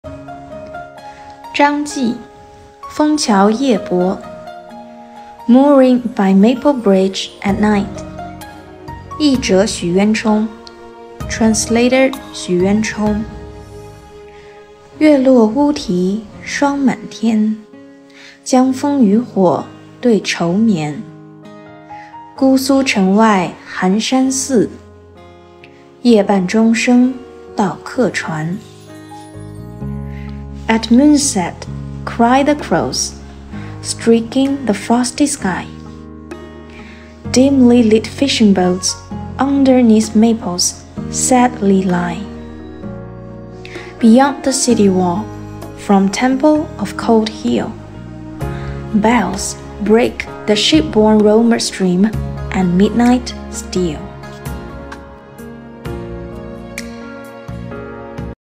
枫桥夜泊 Mooring by Maple Bridge at Night
Embark on a poetic journey with WhisperedVerses as we present "Mooring by Maple Bridge at Night," a timeless Tang dynasty poem beautifully translated by Xu Yuanchong. Immerse yourself in the serenity of Chinese culture and the eloquence of Tang poetry.

【唐】 张继  Zhang Ji
（译者） 许渊冲 Translator Xv YuanChong
月落乌啼霜满天
江枫渔火对愁眠
姑苏城外寒山寺
夜半钟声到客船
At moonset cry the crows
streaking the frosty sky
Dimly lit fishing boats
underneath maples sadly lie
Beyond the city wall
from temple of cold hill
Bells break the ship-borne roamer's dream and midnight still.